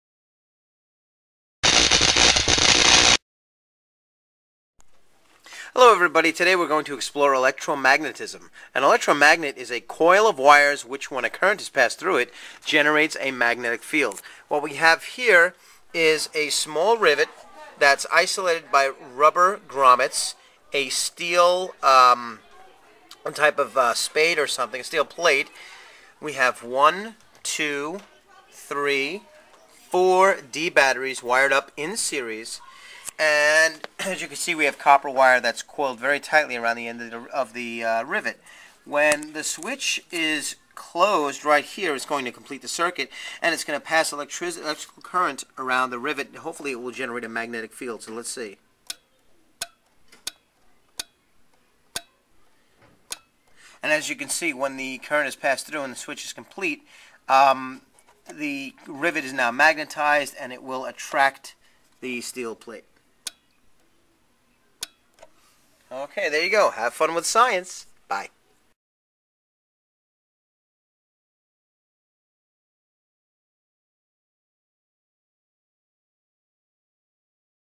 Electromagnetism
In this video, I built an electromagnet.  It is a switched on/off device like a telegraph.   If you look closely, you can see the electromagnet 'turn on' with the switch and attract the metal to the rivet.

I hope that you enjoy watching it!